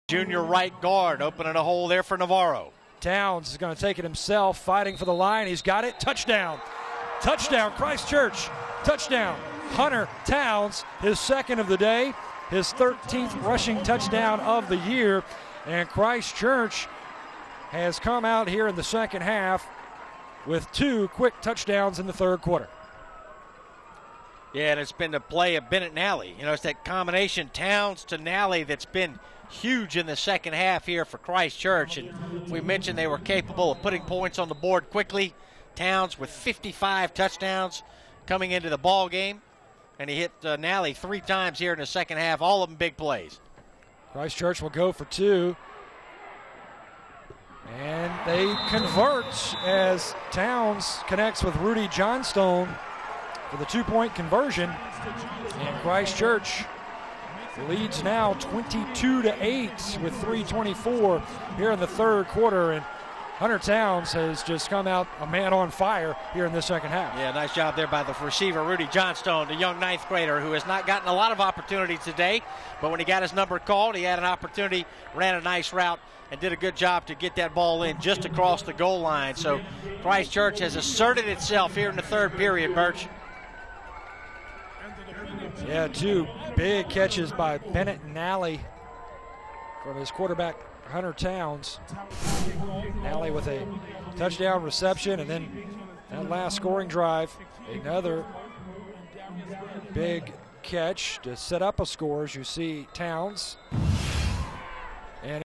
Hunter Townes rushes for a 1 yard touchdown towards the end of the 3rd quarter
In the 2011 SCHSL Class A Division II State Football Championship played in Columbia, SC Hunter Townes sneaks the ball in the end zone for a 1 yard touchdown to put Christ Church up 22-8 with 3:23 left in the 3rd quarter.